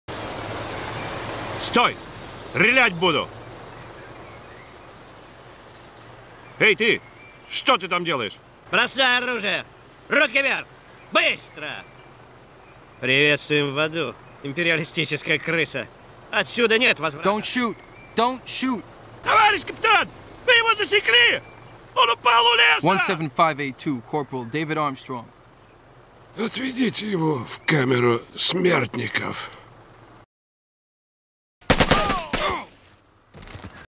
Отведите его в камеру смертников
Жесткие парни отправляют малютку в "камеру смертников"